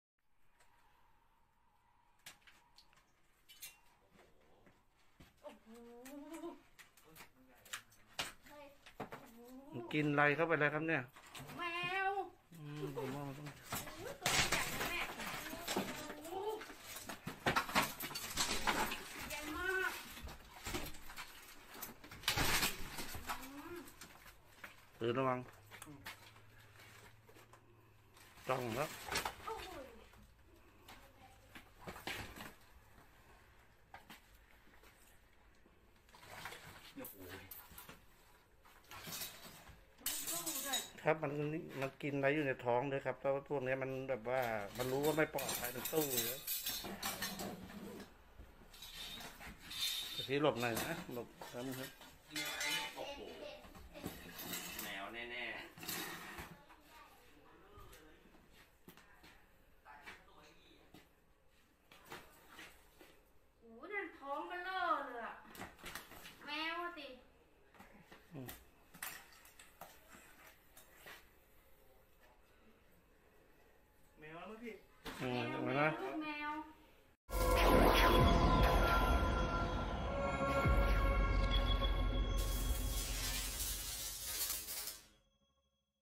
മച്ചിന് മുകളില്‍ കൂറ്റന്‍ പെരുമ്പാമ്പ്;ഞെട്ടി വീട്ടുകാര്‍, ദൃശ്യങ്ങള്‍ | Snake In The Ceiling
A giant snake in the ceiling viral video.
മച്ചിന് മുകളില്‍ കൂറ്റന്‍ പെരുമ്പാമ്പ്; വലിപ്പമറിഞ്ഞത് സീലിങ് പൊളിഞ്ഞപ്പോള്‍, ഞെട്ടി വീട്ടുകാര്‍, സാഹസികമായി പിടികൂടി പാമ്പുപിടിത്തക്കാരന്‍. സാമൂഹ്യമാധ്യമങ്ങളില്‍ വൈറലായി ദൃശ്യങ്ങള്